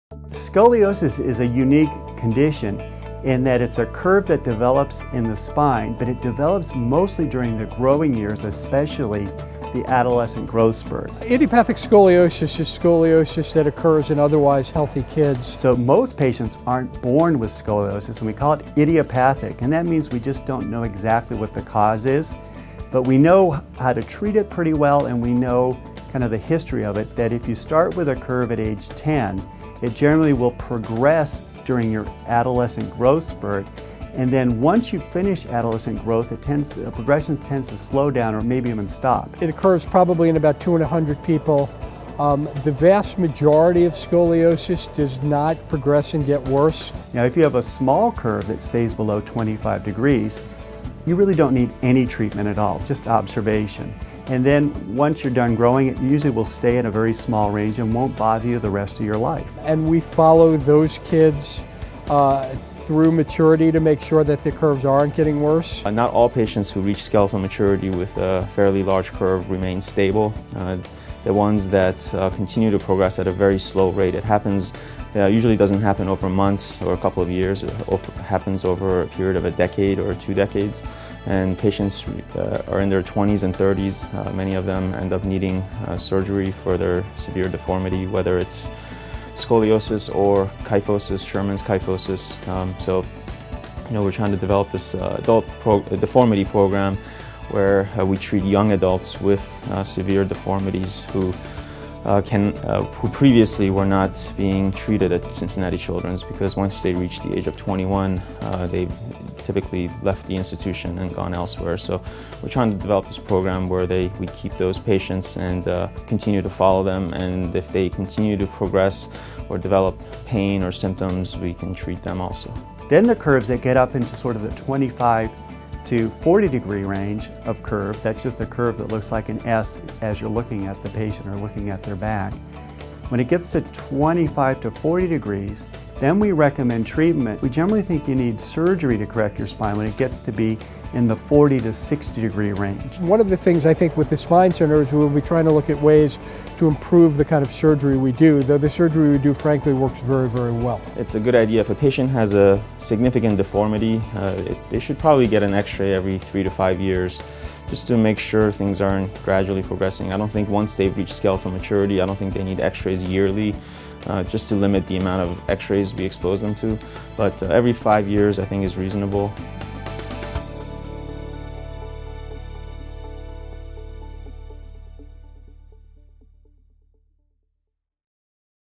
Adolescent Idiopathic Scoliosis
Experts from the Crawford Spine Center at Cincinnati Children's discuss the diagnosis, treatment and long term outlook for patients with adolescent idiopathic scoliosis.